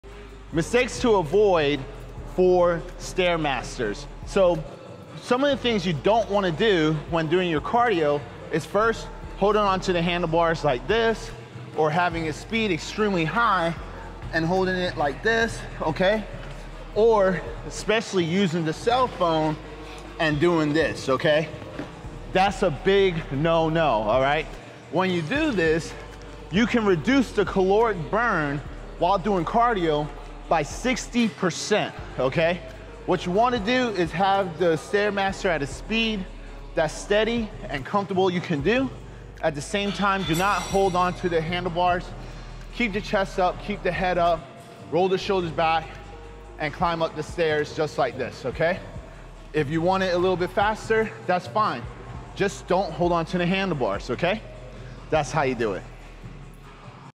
Mistakes To Avoid For Stairmasters
Mistakes to AVOID on the Stair Master ❌😬

I know it sounds hard to make mistakes on the stair master…

… but I see it all the time.

The first thing you’re doing wrong is with your hands:

❌ Don’t hold the handles

❌ Don’t hold the top of the screen

❌ Don’t hold your phone

This reduces the caloric burn by 60% and you’re now not getting enough out of the workout.

What you want to do is have the stairmaster at a comfortable, steady pace, keep the chest and head up with rolled back shoulders and climb slowly.

If you want to increase your speed a bit that’s fine, but don’t hold on to anything.

If you liked this video, comment a 🔥🔥below and I’ll make more!